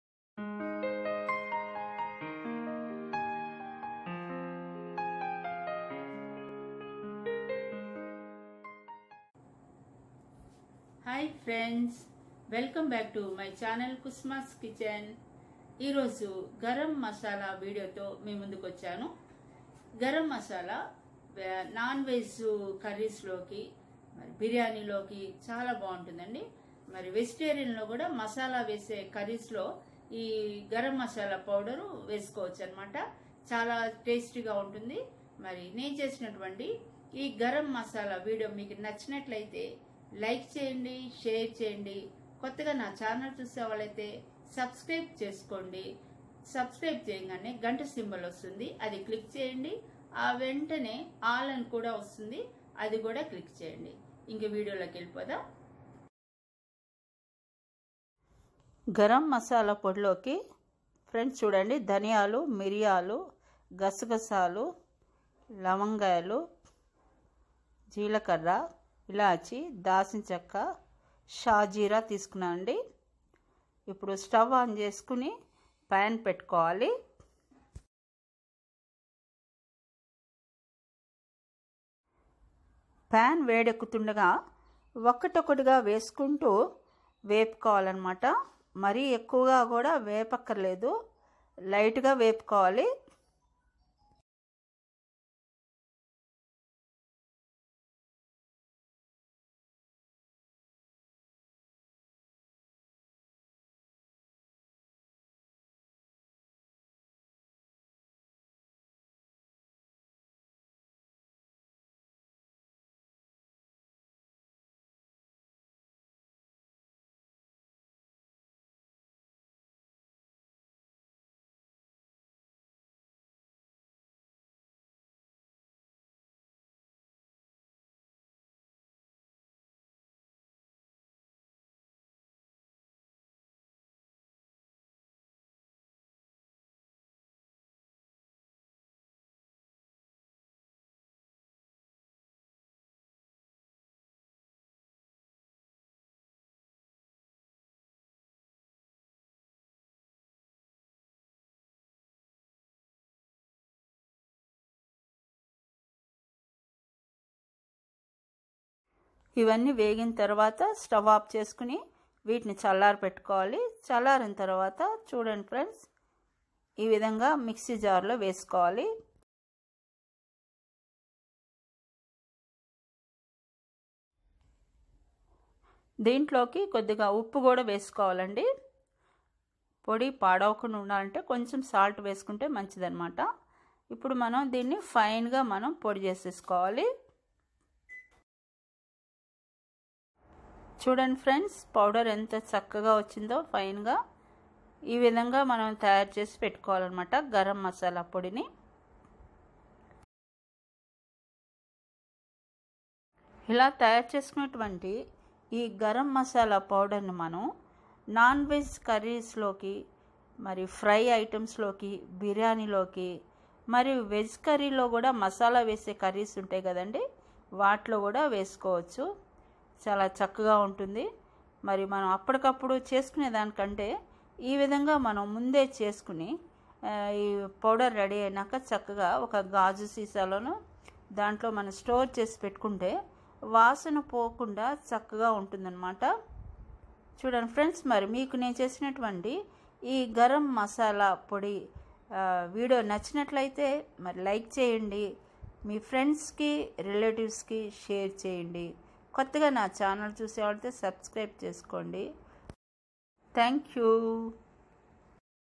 ఇంట్లో చేసుకునే గరం మసాలా // HOW TO MAKE GARAMMASALA POWDER // KUSUMAS KITCHEN
ఈ మసాలాపౌడర్ నాన్ వెజ్ కర్రీ, ఫ్రై, బిరియాని లోకి, వెజ్ మసాలా కూరలకి చాలా బాగుంటుంది